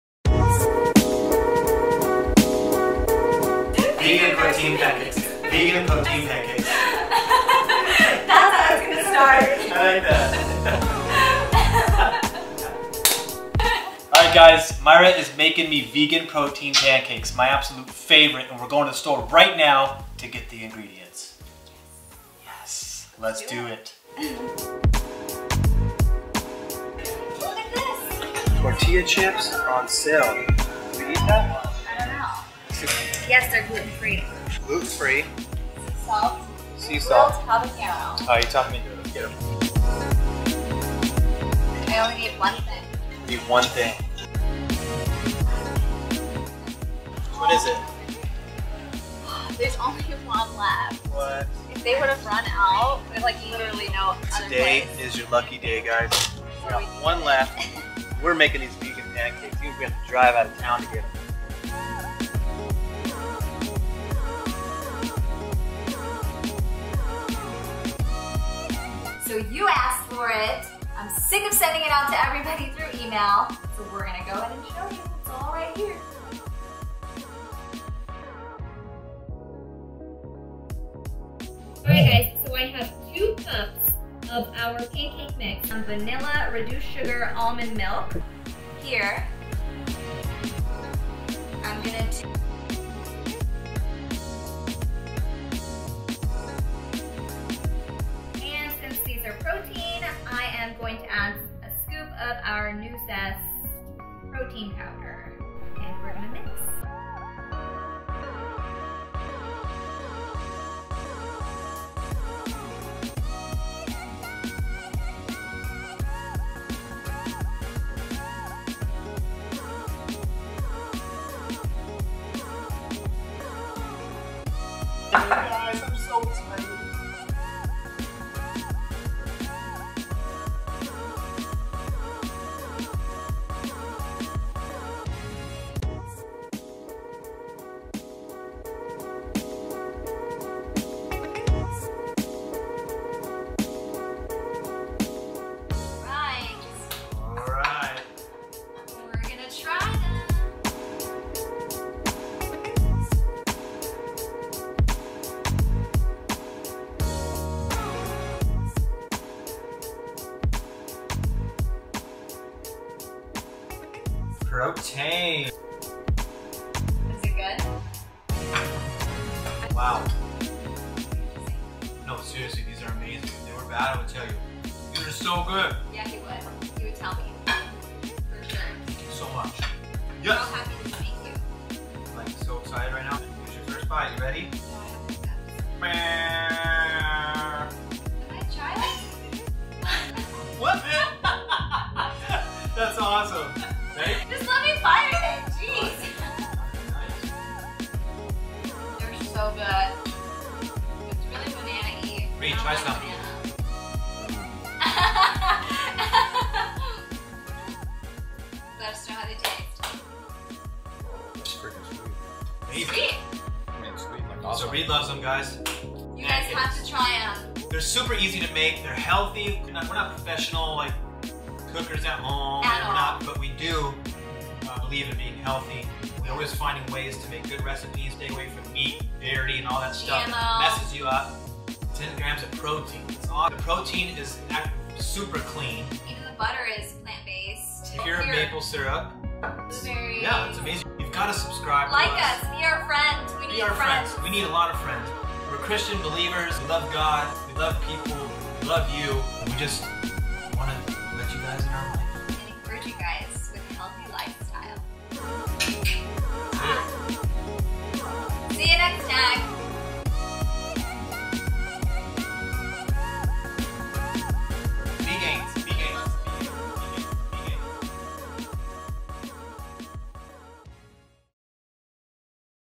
HOW TO MAKE HEALTHY VEGAN PROTEIN PANCAKES !
In this video we show you how to make healthy vegan protein pancakes.

No dairy, or any junk. All very healthy stuff. Recipe Below. Enjoy!

You can use any protein you want, but we find this to be the cleanest and healthiest without any chemicals or whey.